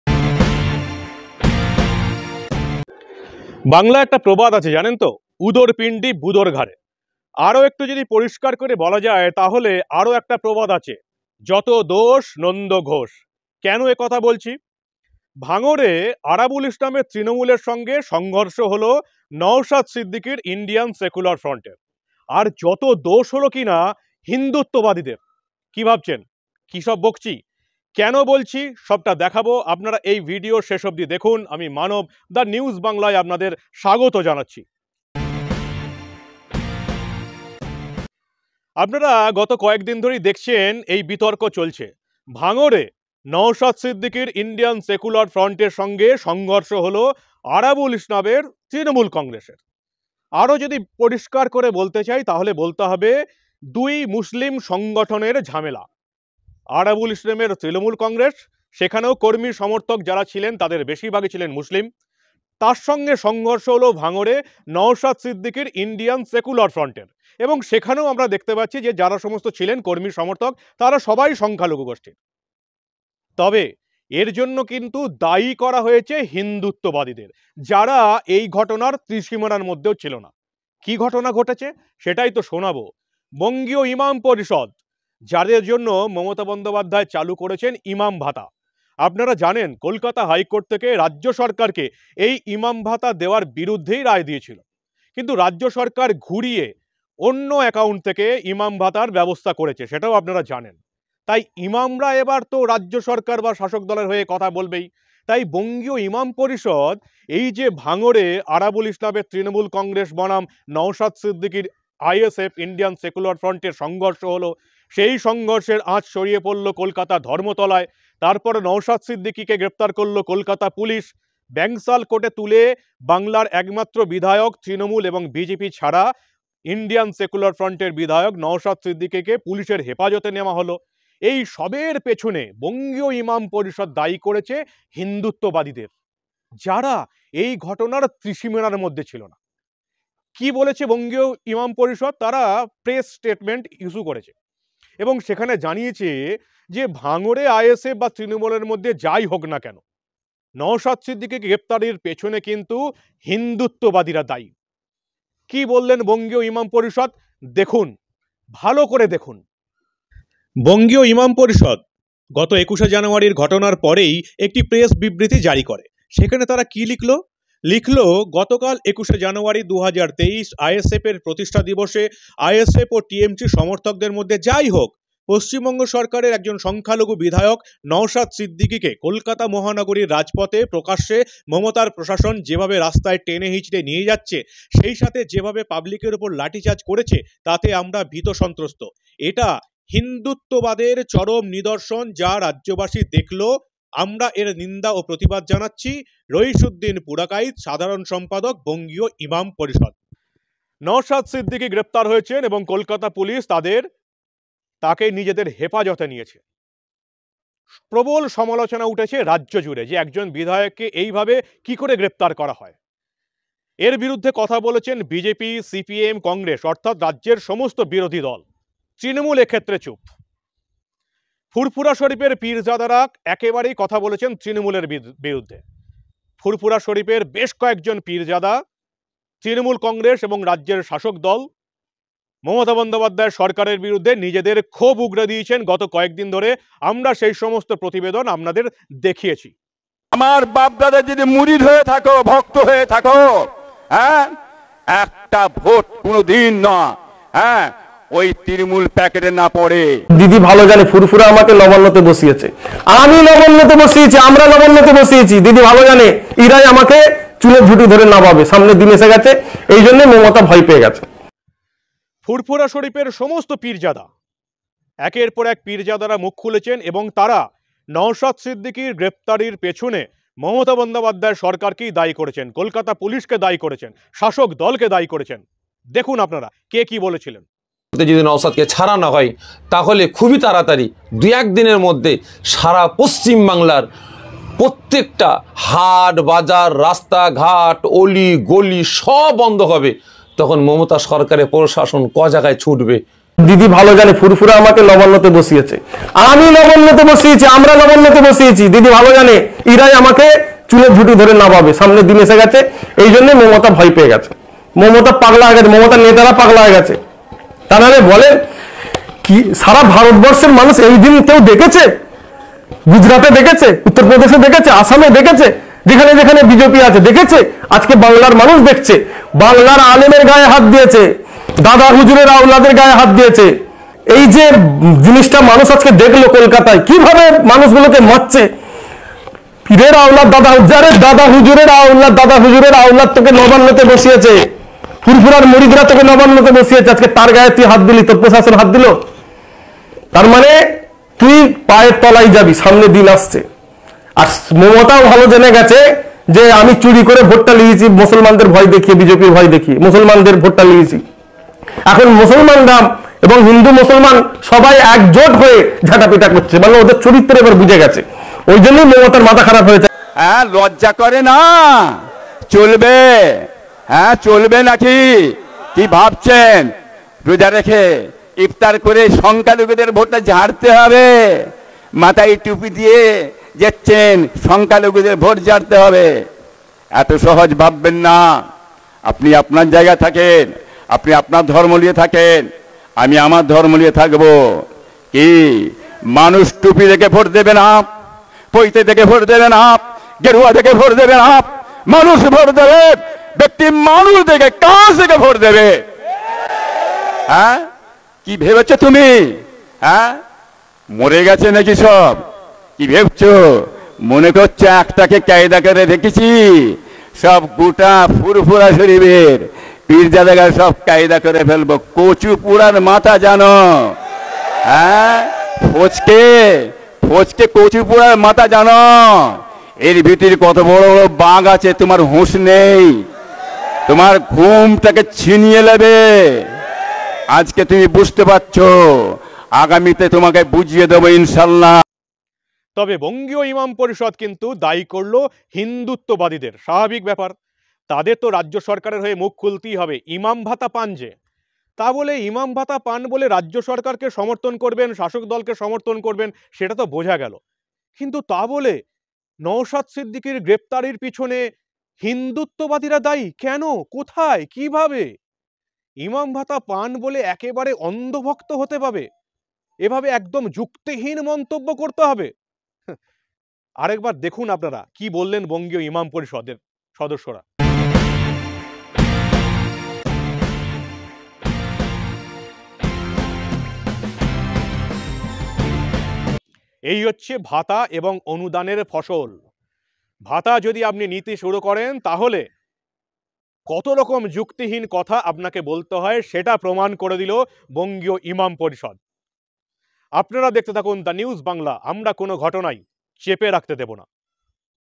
সরকারি ভাতায় অন্ধ ইমামদের কীর্তি হিন্দুত্ববাদীদের বিরুদ্ধে
এলাকার লড়াই আরাবুল নৌশাদের, দোষ হিন্দুত্ববাদীদের @thenewsbangla